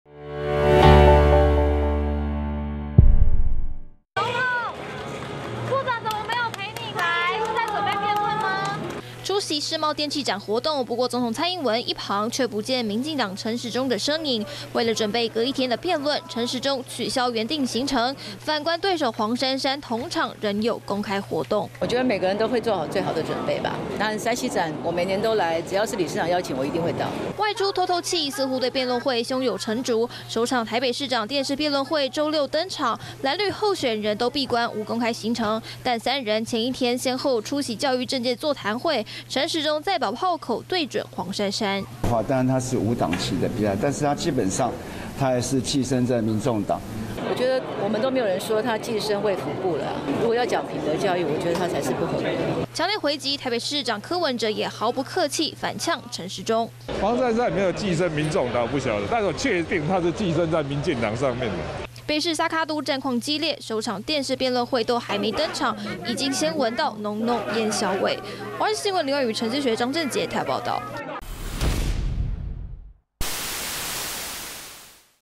寄生民眾黨? 珊珊反酸阿中"寄生衛福部"｜華視新聞 20221104
首都市長選戰激烈，各陣營候選人本週都開始密集訓練，紛紛閉關沒有公開行程，今(4)日只有黃珊珊仍外出透透氣，面對民進黨台北市長候選人陳時中前一天，嗆她寄生在民眾黨，黃珊珊則回擊，沒說他寄生衛福部，而台北市長柯文哲今日上午表示，確定陳時中是寄生在民進黨。